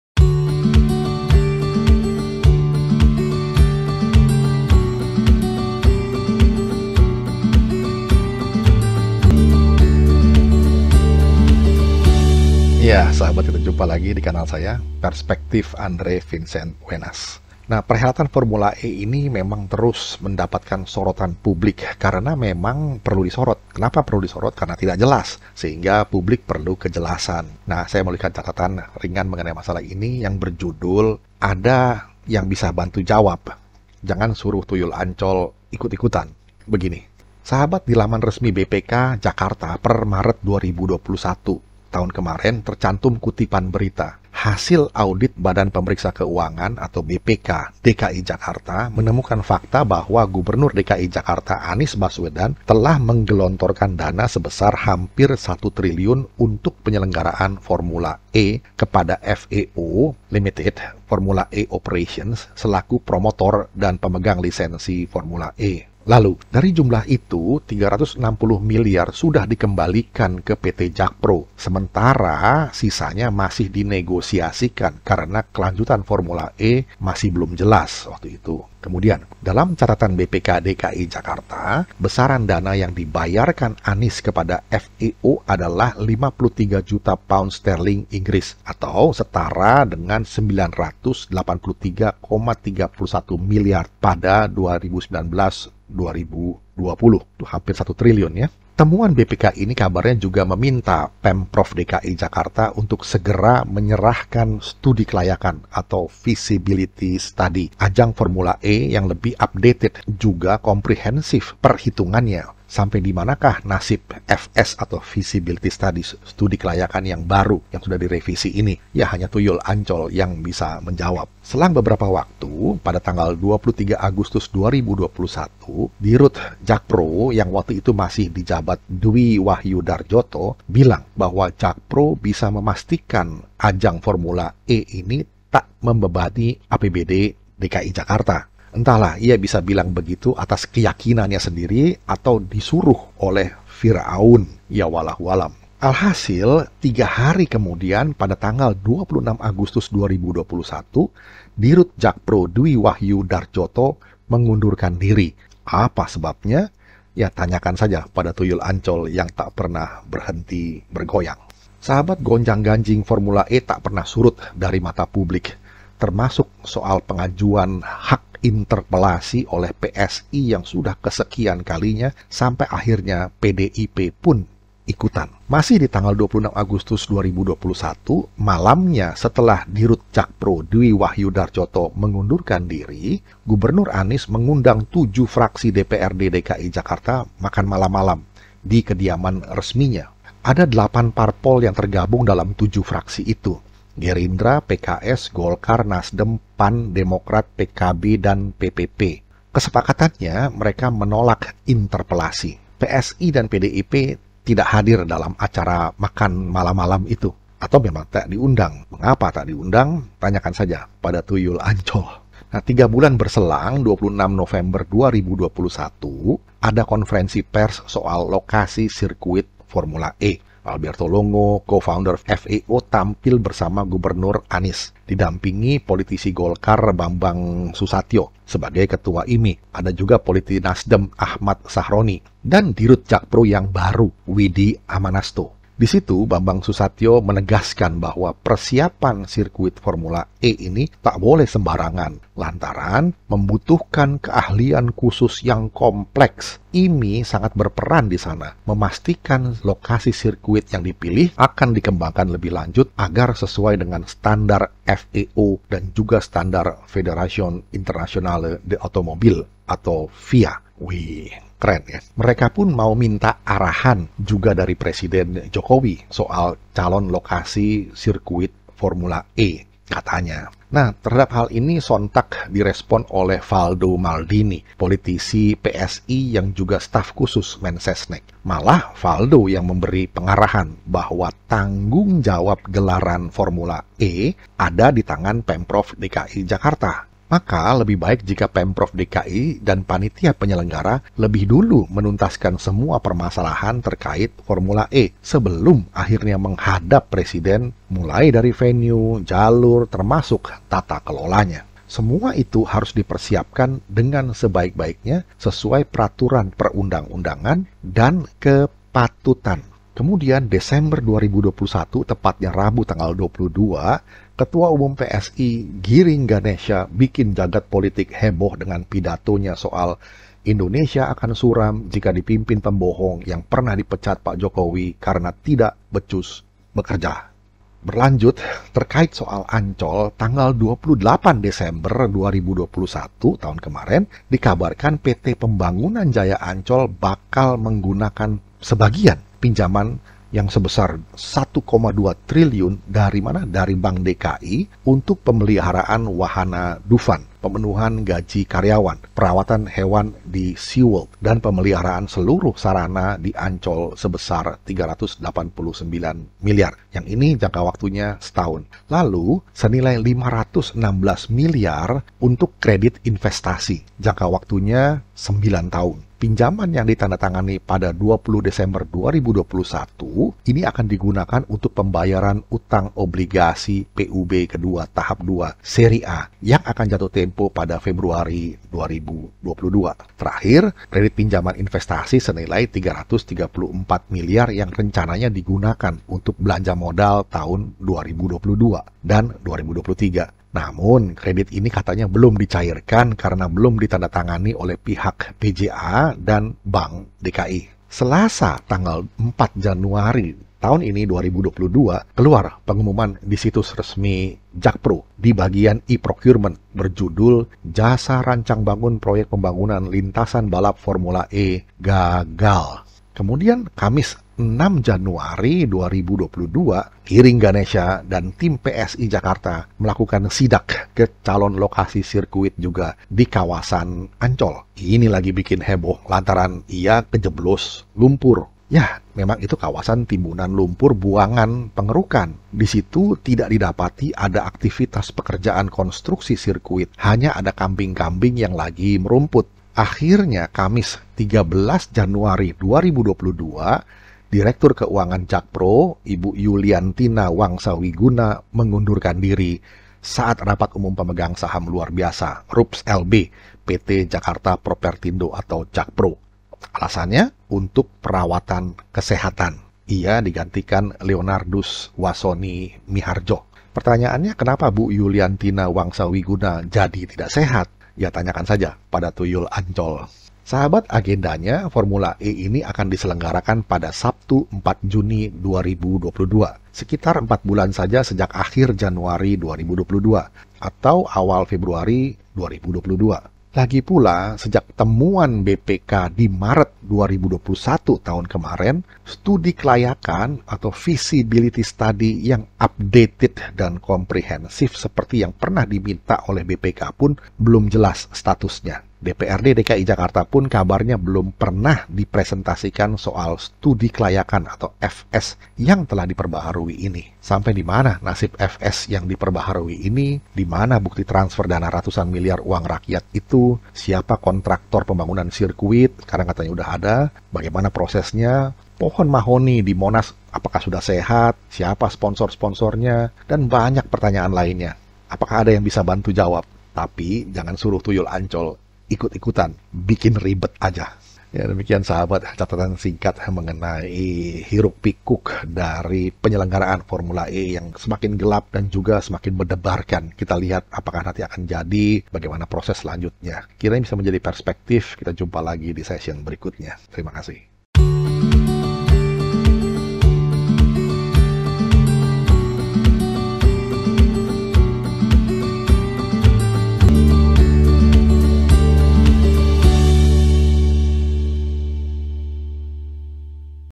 ADA YANG BISA BANTU JAWAB ?? JANGAN SURUH TUYUL ANCOL IKUT-IKUTAN !!
Sampai dimana nasib STUDI KELAYAKAN Formula-E? Dimana bukti transfer dana ratusan milyar itu? Bagaimana proses lelang pembangunan sirkuit? Pohon Mahoni di Monas sudah sehat? Dan banyak pertanyaan lainnya. Apakah ada yg bisa bantu jawab?

Andre Vincent Wenas, DRS,MM,MBA.